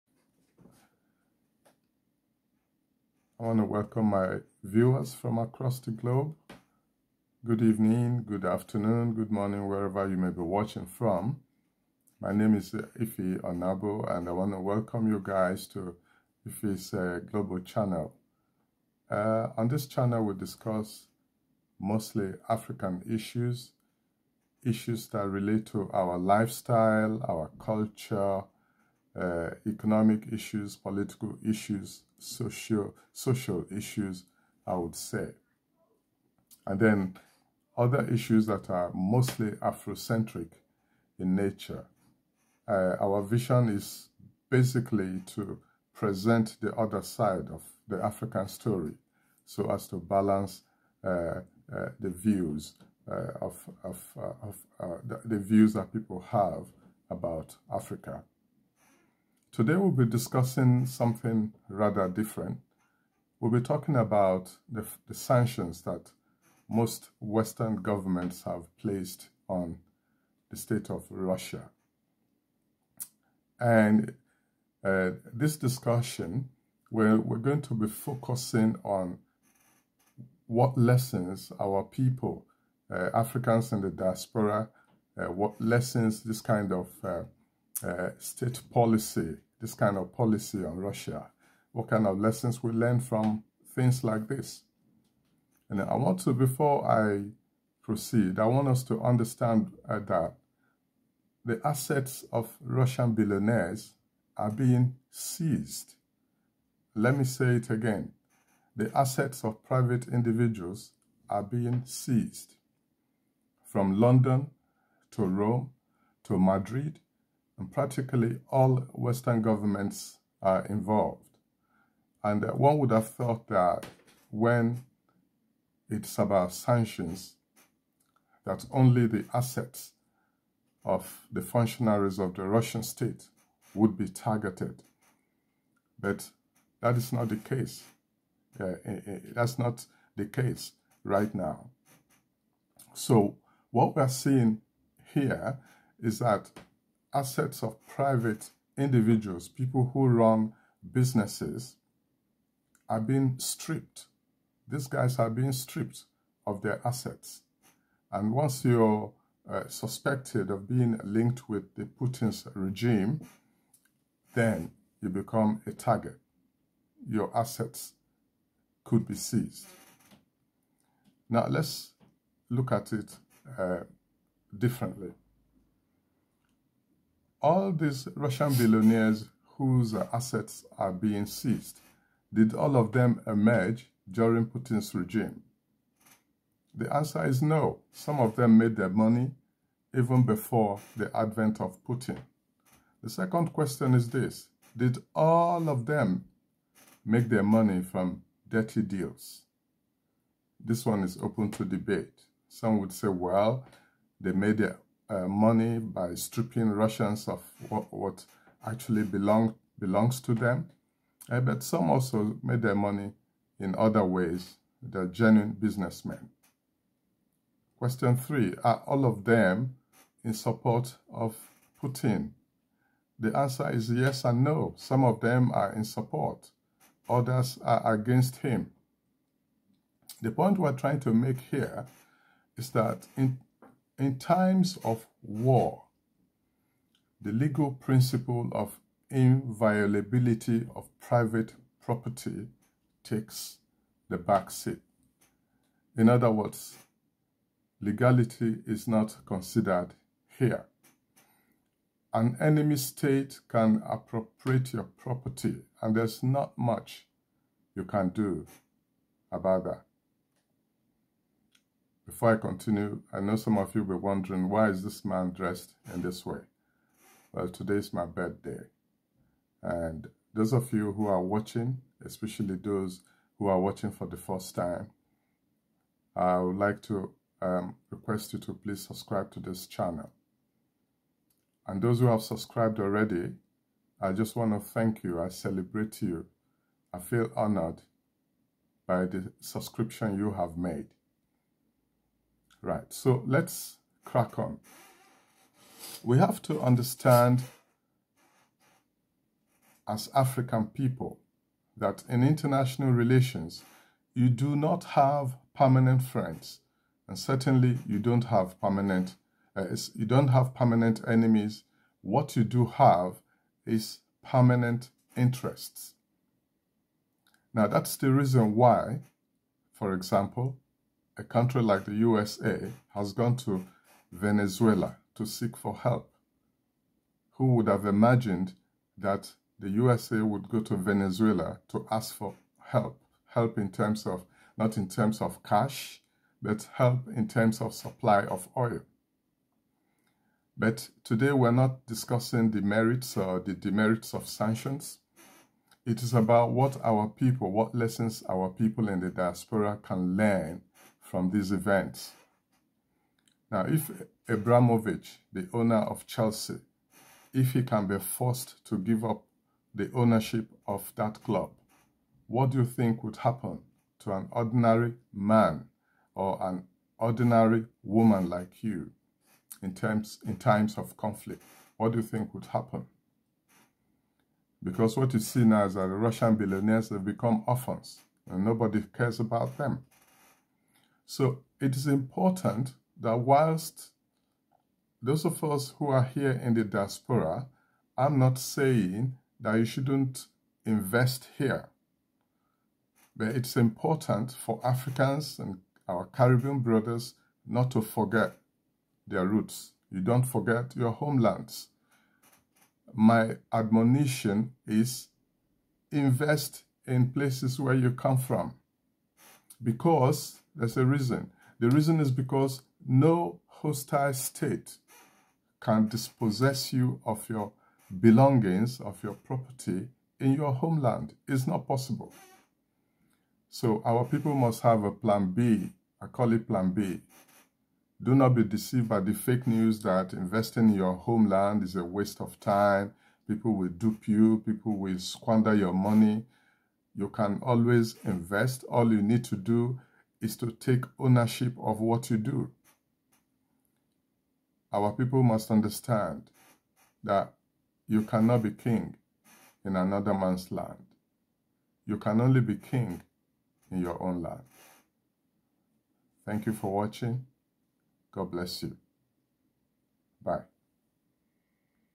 Sanctions: What Lessons for Africans in the Diaspora?
This video analyses the regime of sanctions placed on the Russian Federation by the West.

But the assets of private citizens of Russia are being seized across the Western world.

This means that not only the functionaries of the Russian state are being targeted, but also private individuals.

The lesson we learn as Africans is that it is advisable to invest in our homeland or the communities where we originate from.

That way, you avoid or minimise the risk of your assets being seized by foreign hostile states in times of war.

Suggestions are also welcome.

Thanks.